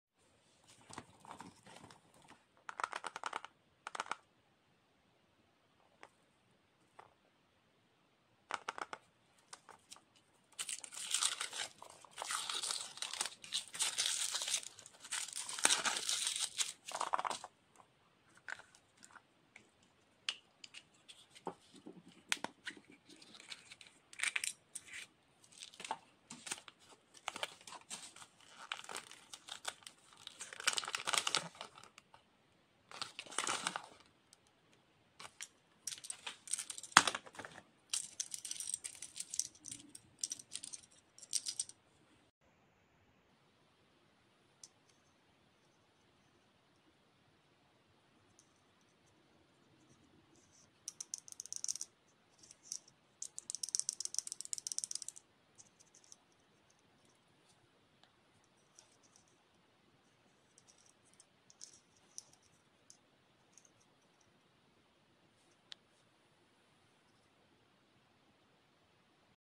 100 Kinder Surprise Eggs | kinder surprise natoons | A Lot Of Candy | ASMR Satisfying video
Hi!!! 😊😊😊
Today we're opening Surprise Eggs - Kinder Surprise Natoons asmr